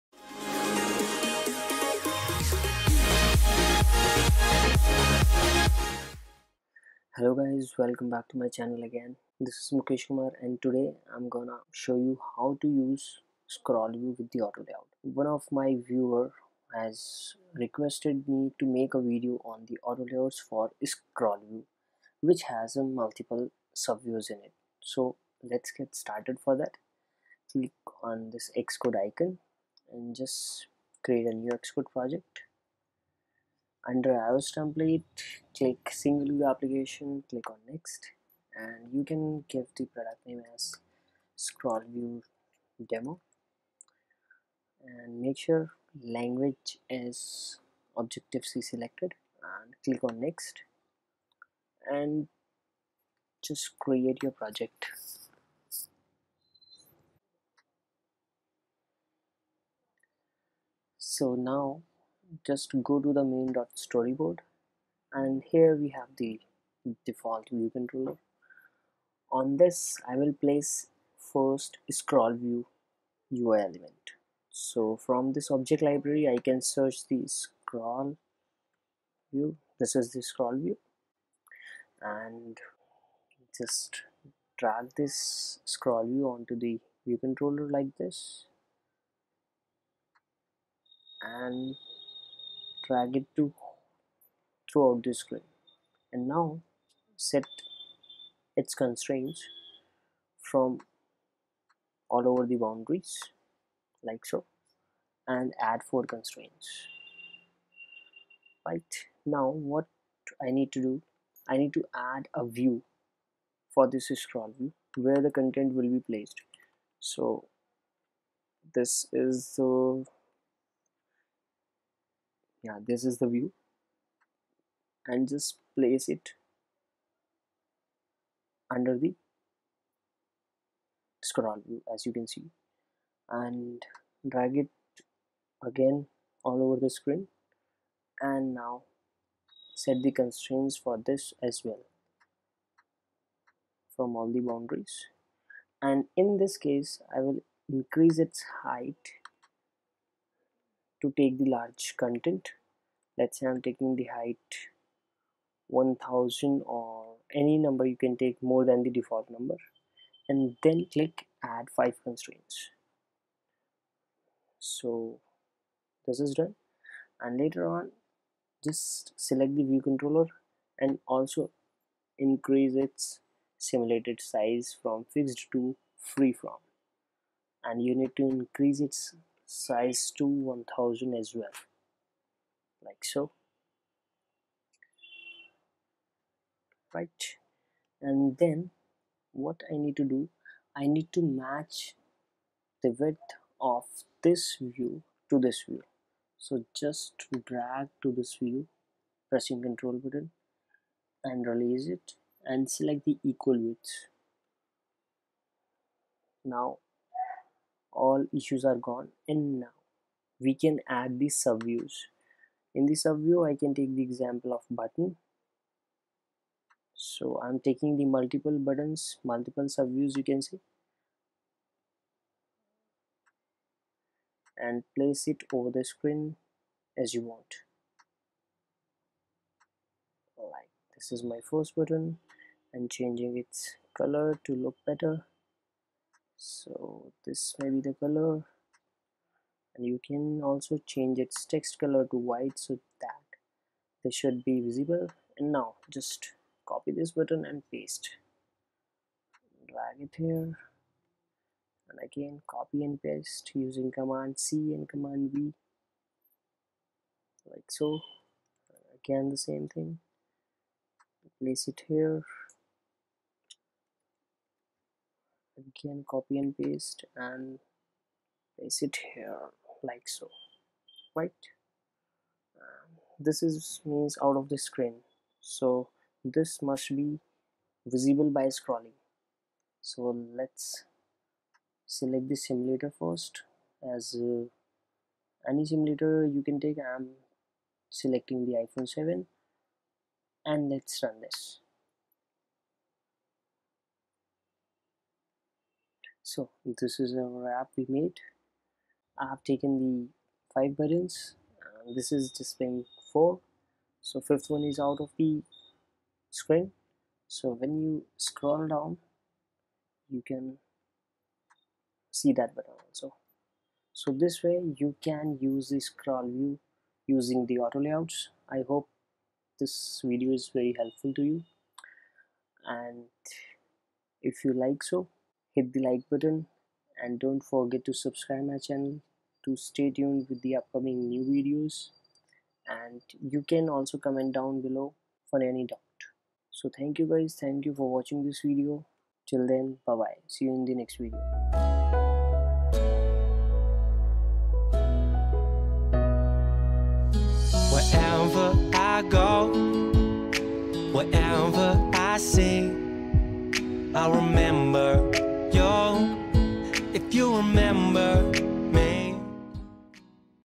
How to use ScrollView with AutoLayout in iOS using Xcode8 ?
In this video, I have shown the use of Scroll View with Auto Layout in iOS using Xcode 8.